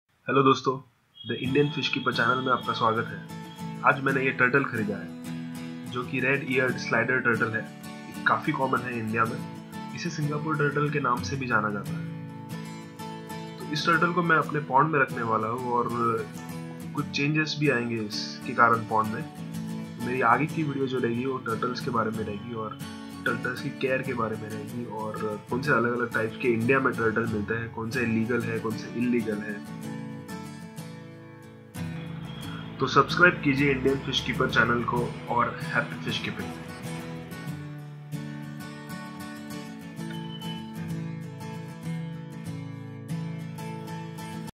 I bought A Turtle
I just bought a new turtle. Its a Red Eared Slider turtle, commonly known as Singapore turtle.